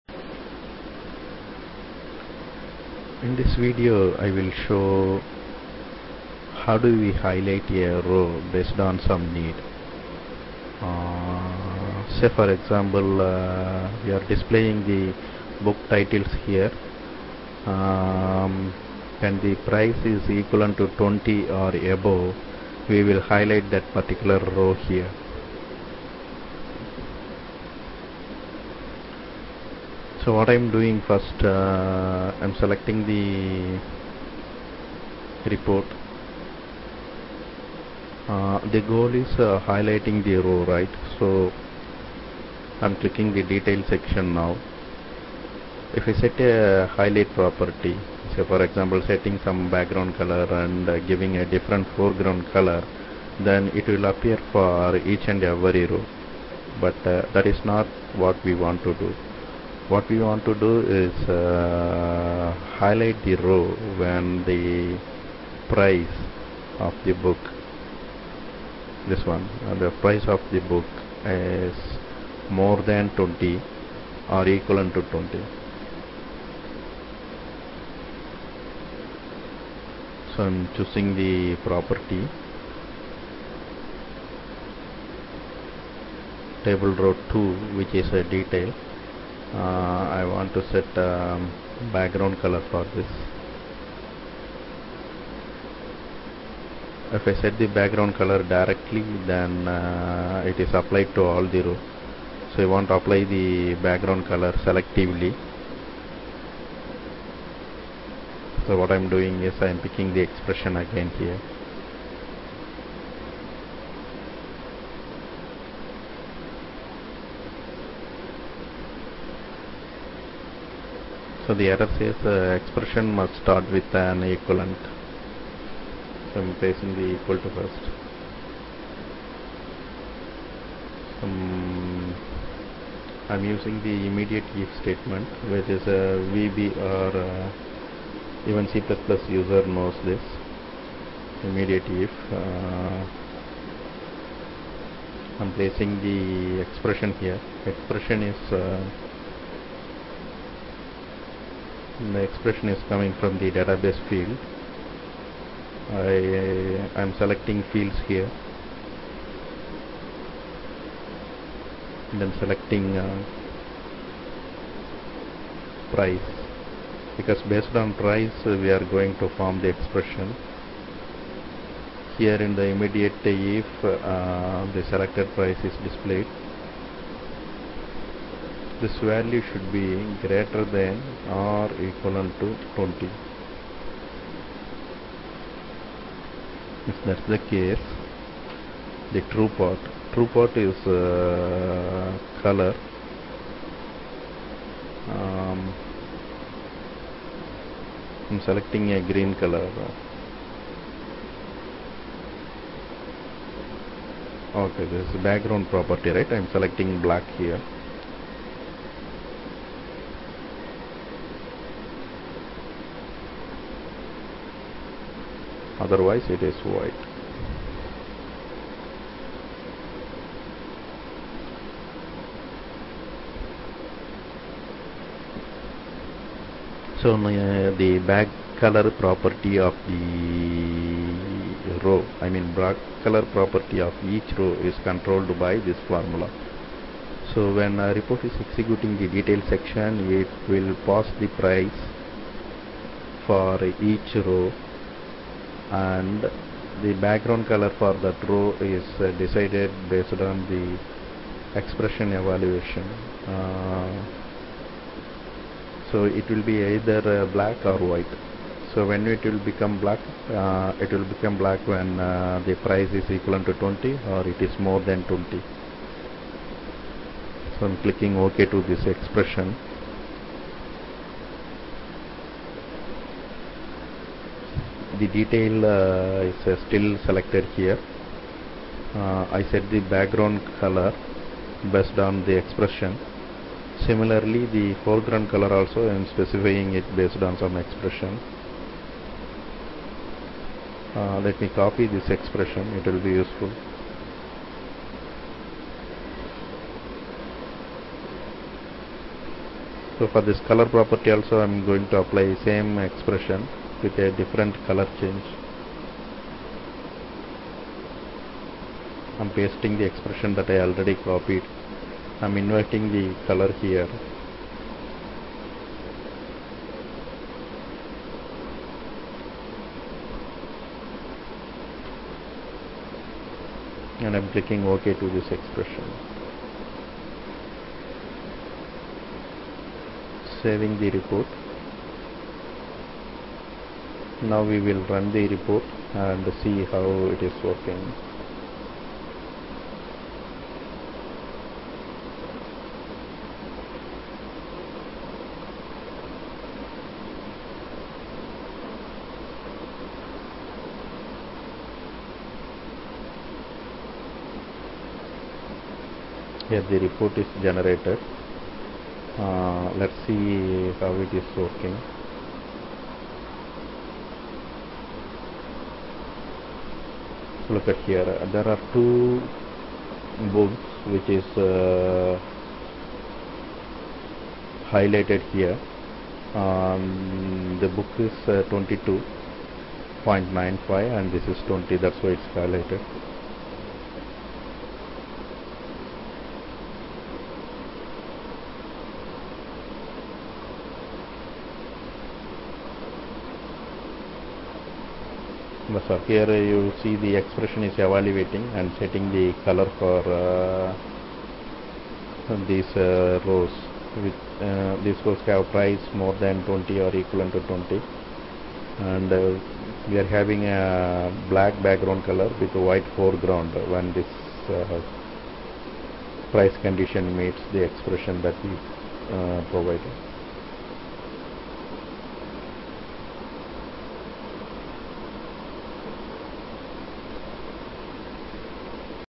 05DbField_Expression
Forming expression based on the database field and using that in report template